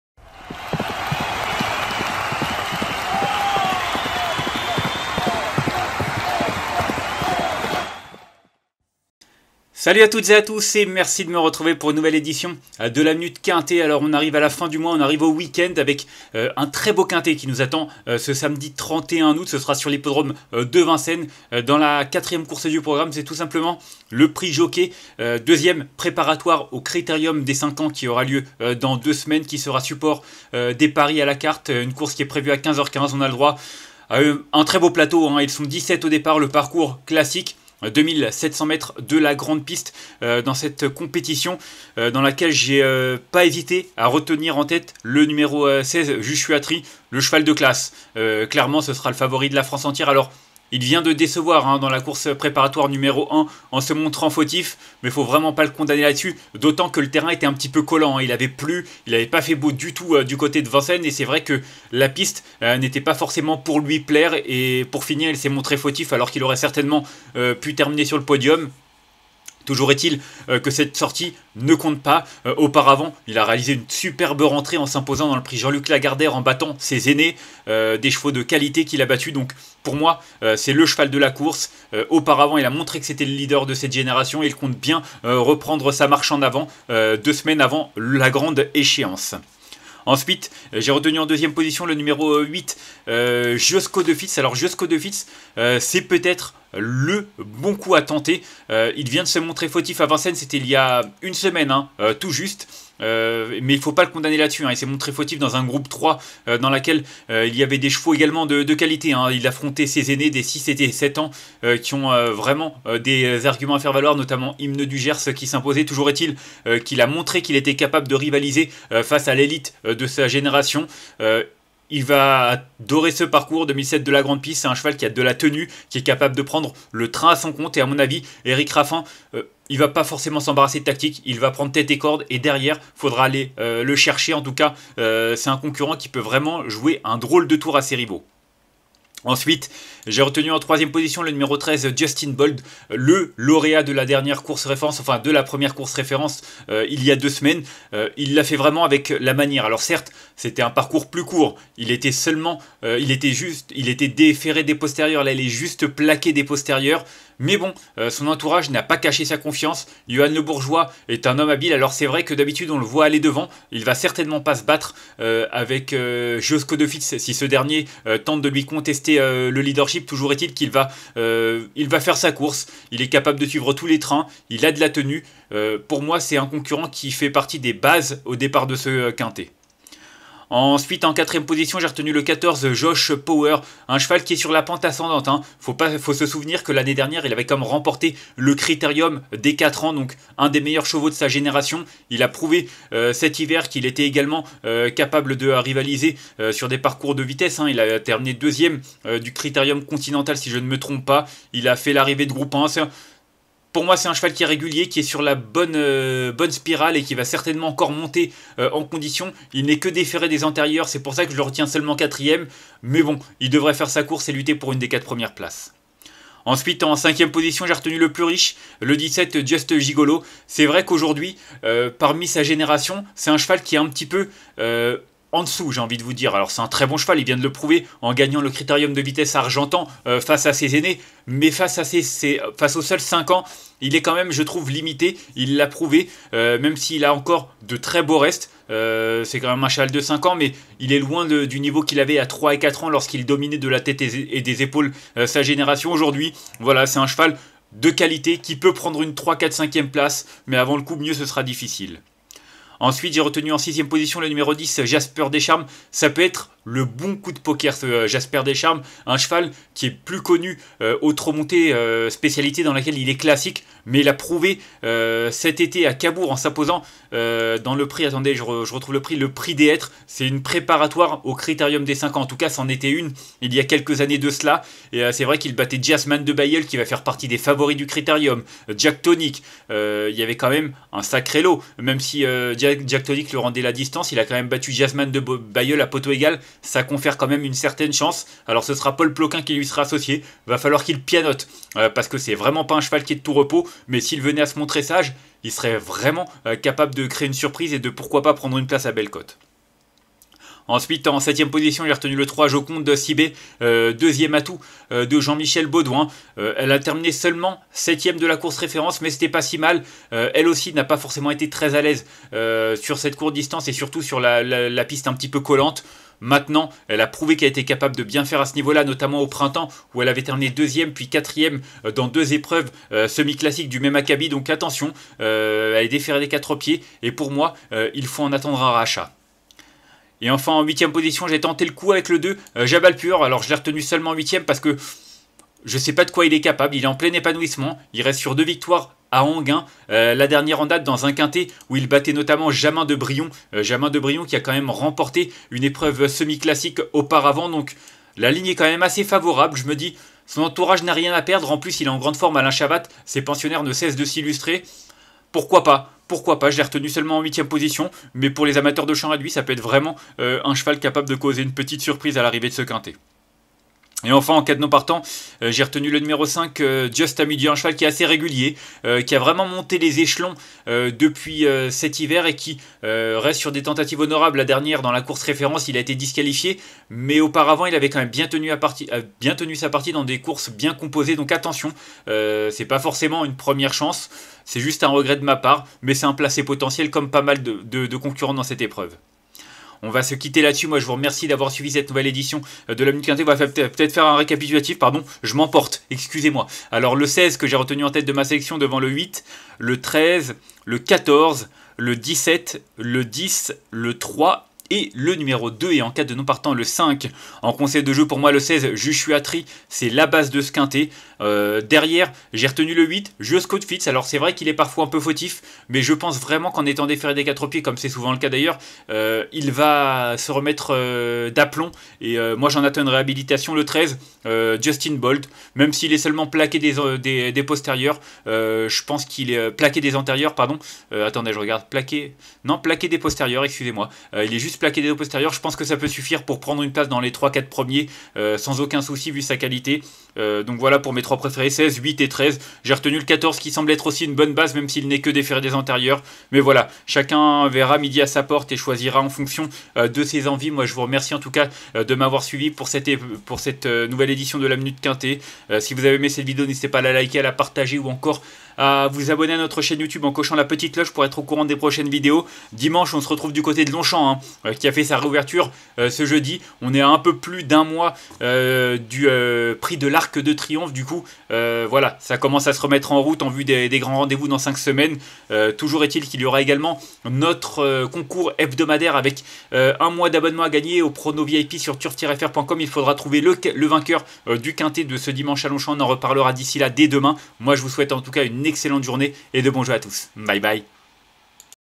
Pronostic Quinté du jour PMU samedi 31 août 2024 à Vincennes #336528
L'élite des trotteurs de 5 ans se retrouvent pour un dernier round avant le grand rendez-vous. Les favoris devraient se tailler la part du lion. Indice de confiance: 4/5.